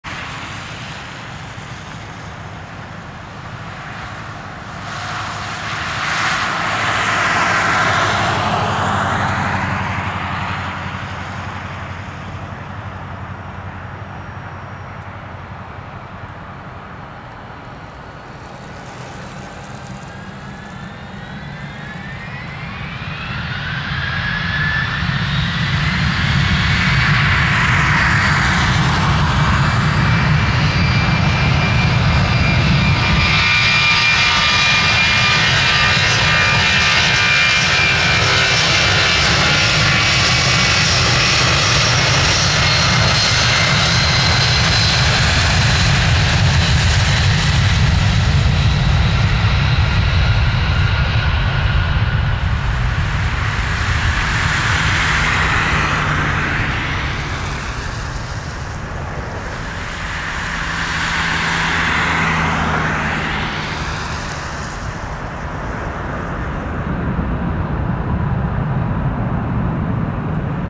Delta Air Lines N624AG 757-200 Takeoff Portland Airport (PDX)
Flight: DAL773 PDX-ATL